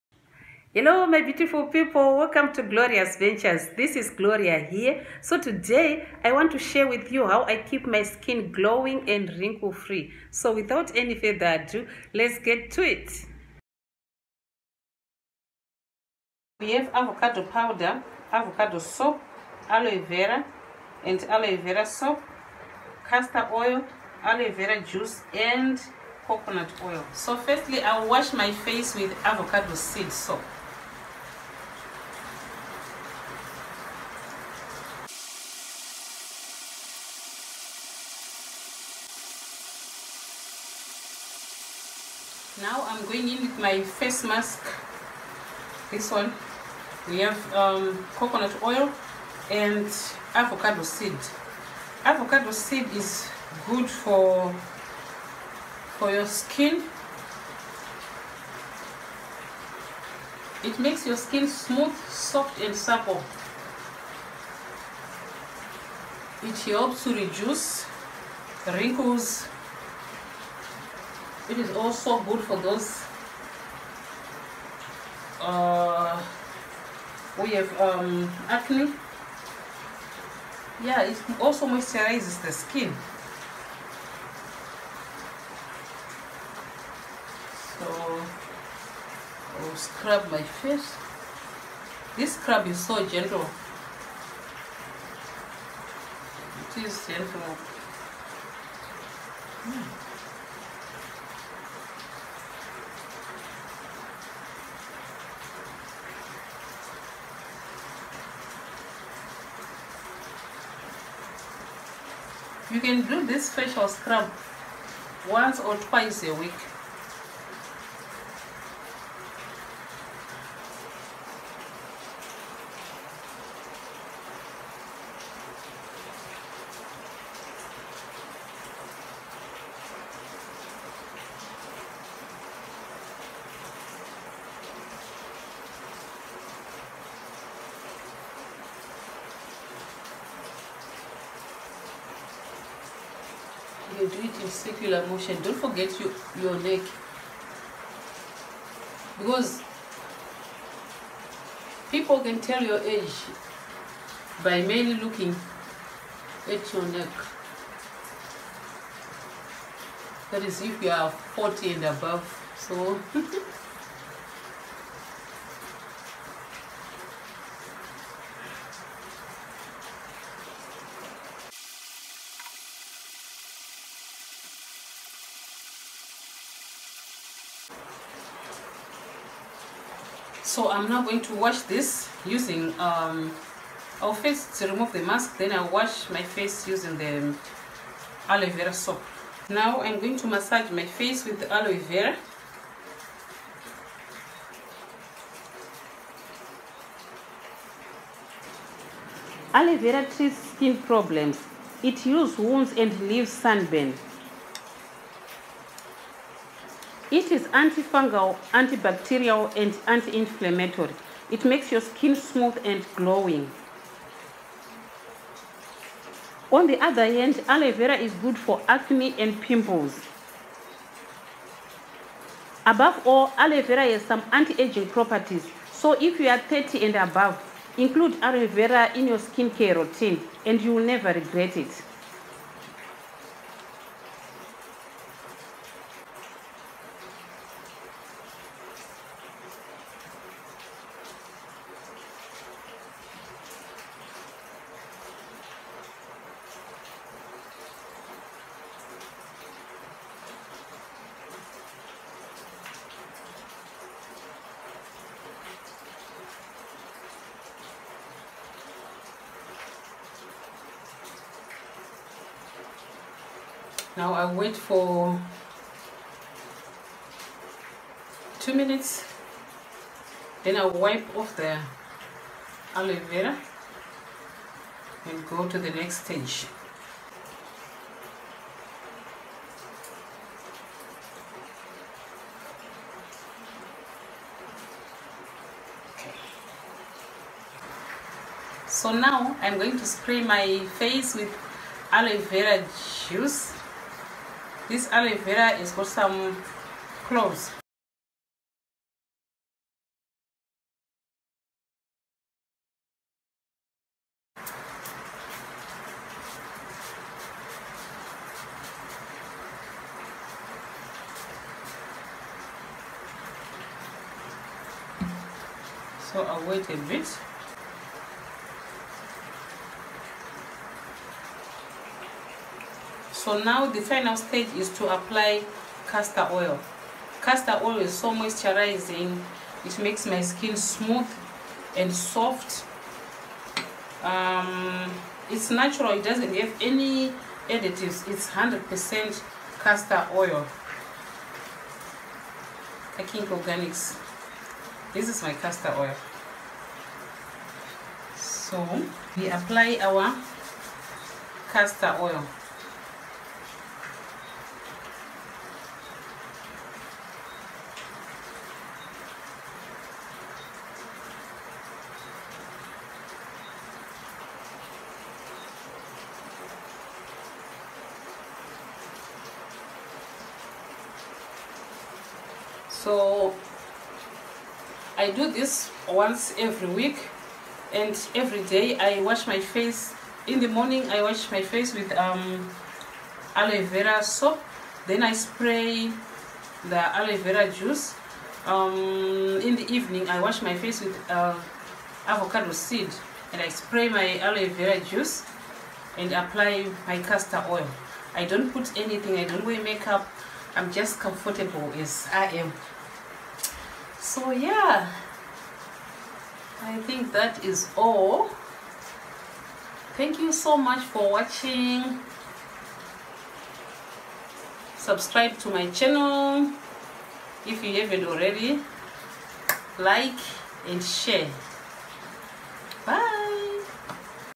How To Use Aloe Vera For A Flawless Skin / Aloe Vera For A Clear Glowing Skin
Hello, my beautiful people. In today’s video, I am going to show you how I keep my skin soft, smooth, moisturized, and wrinkle free. I will be using all natural products. Everything I will be using is natural and handmade.  The aloe vera is from my backyard, the soaps and oils I made them. I don’t use products with added colours and perfumes because I am allergic to most of the store bought things.
 Hope this will help someone among us. Thank you, and stay blessed ❤️🙏🏾